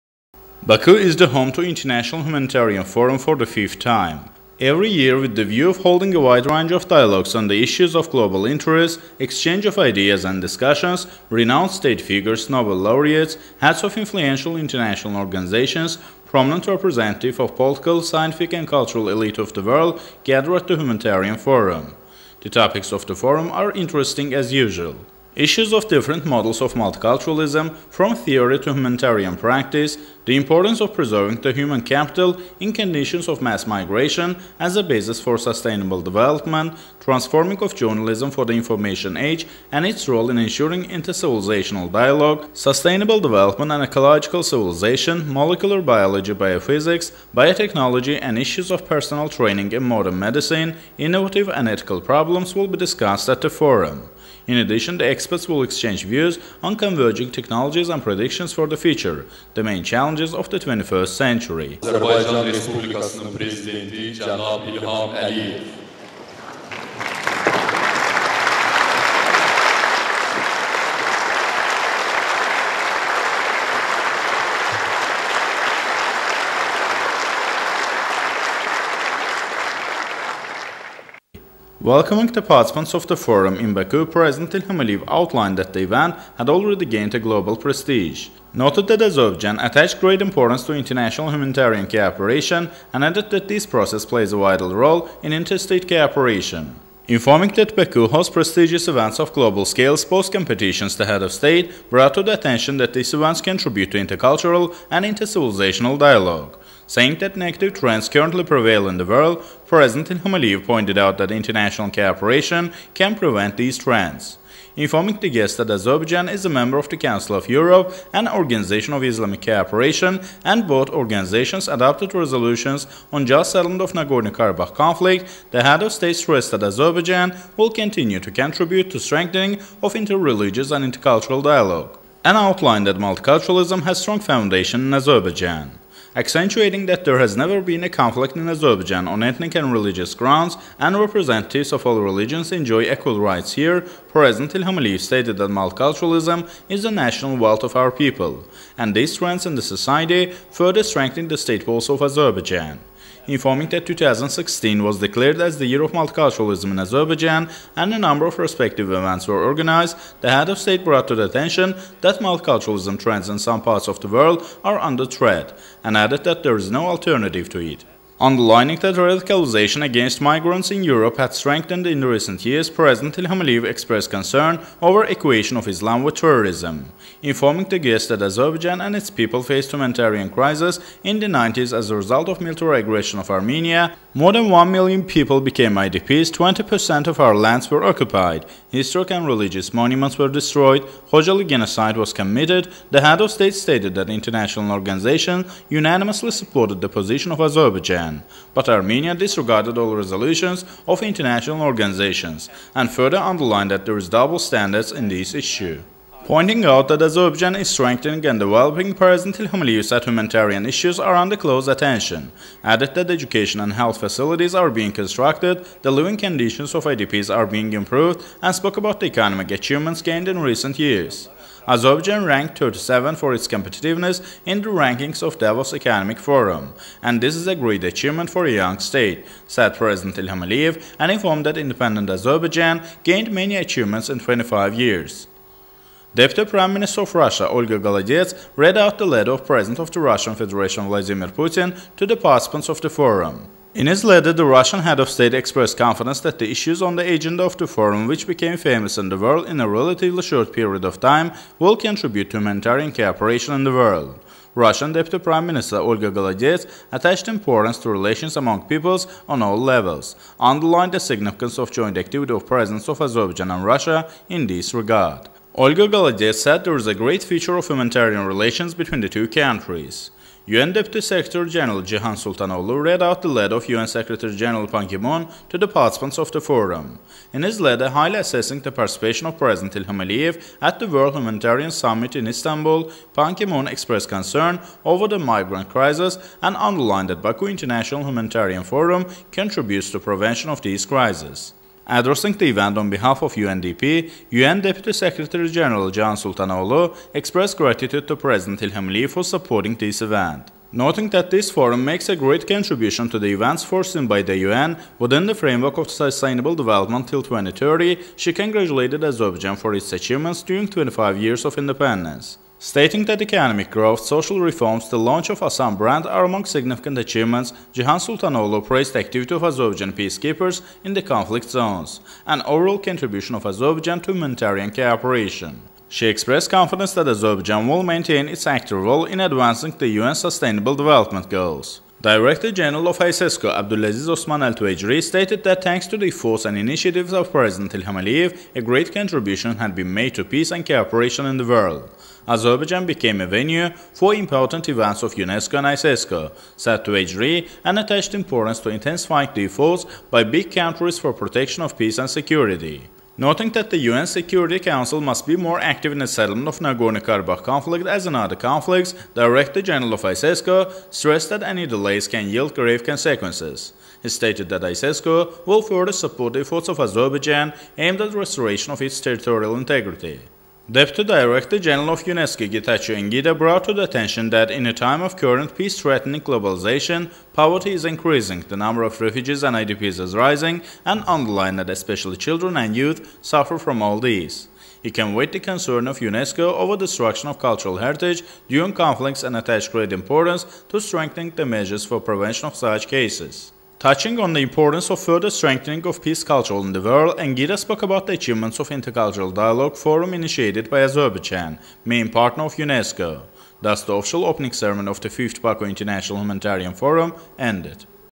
5th International Humanitarian Forum is launched in Baku
The 5th Baku International Humanitarian Forum started, September 29, at the Heydar Aliyev Centre.

President of the Republic of Azerbaijan Ilham Aliyev and First Lady Mehriban Aliyeva attended the official opening ceremony.

Head of our State Ilham Aliyev addressed the Forum’s opening ceremony.

Then, deputy head of the Russian Federation’s Government Mrs Olga Golodets read an address by Russian President Vladimir Putin to Forum participants and, making a speech, highly appreciated the significance of the event.